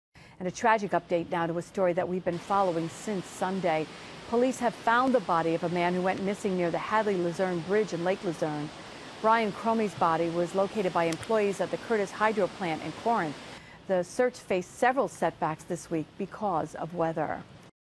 Body of missing man found in Hudson River
LAKE LUZERNE  (AP) -- State police say the body of one of two men who went missing in northern New York rivers over the weekend has been found. They say workers at a hydroelectric plant found the body of 42-year-old Brian Cromie of Massena at about 11:30 a.m. Wednesday. He'd been missing since being washed off a small raft while riding rapids at a waterfall on the Hudson River in the Warren County town of Lake Luzerne on Sunday.  In Washington County, police divers hadn't found any sign of 39-year-old Richard Kelley of Whitehall as of late Tuesday. He was swept underwater while swimming in the Mettawee River with his family Saturday.   AP-WF-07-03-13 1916GMT